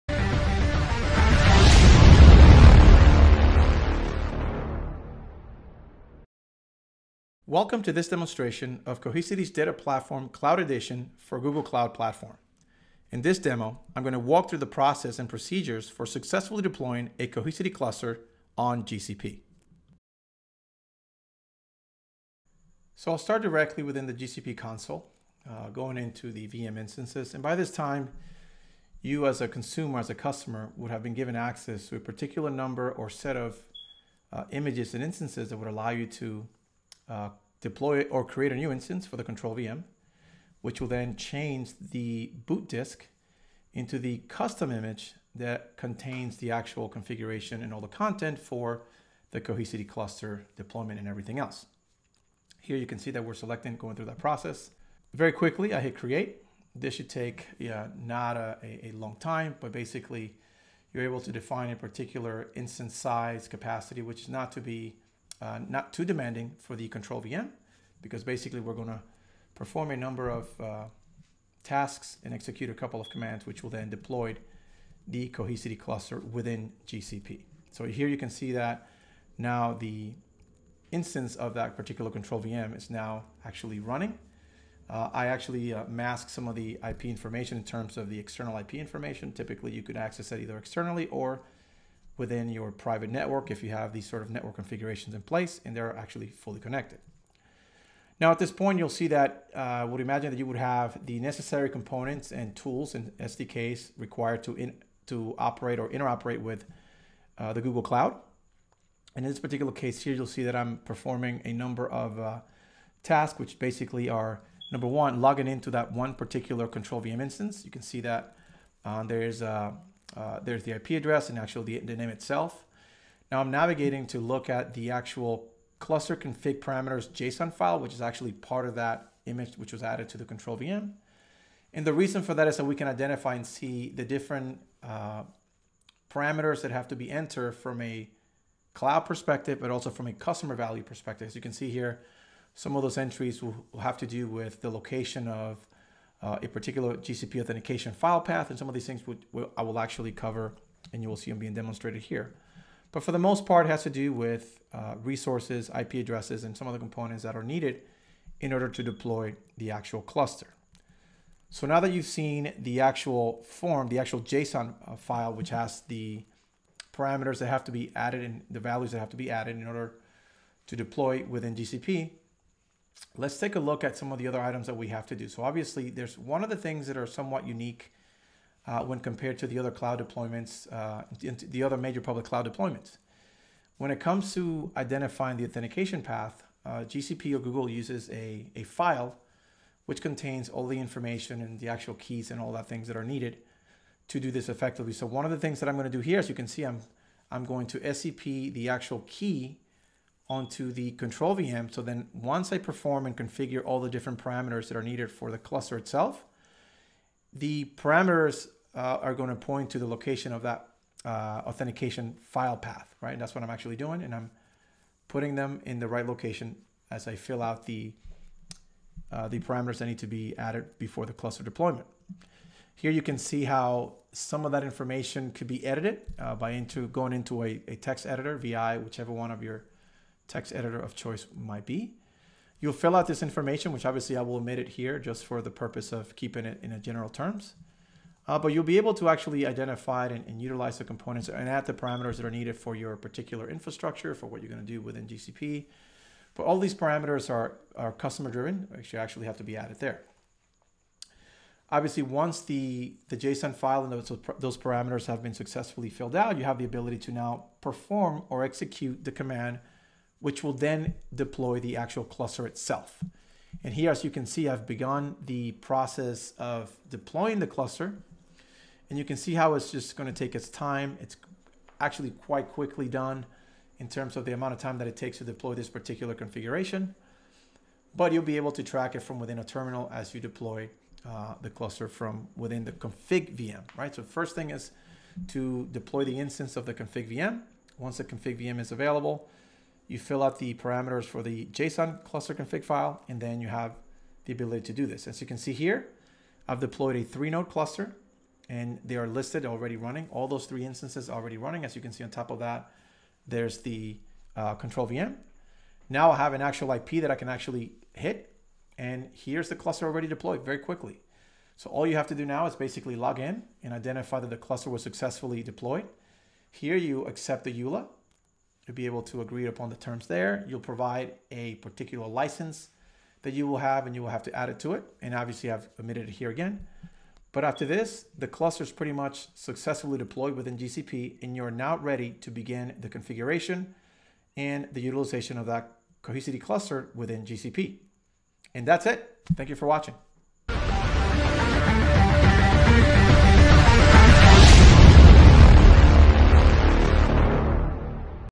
Cohesity Cloud Edition Deployment on Google Cloud Platform
This demonstration showcases the procedure for deploying Cohesity DataPlatform Cloud Edition in Google Cloud Platform.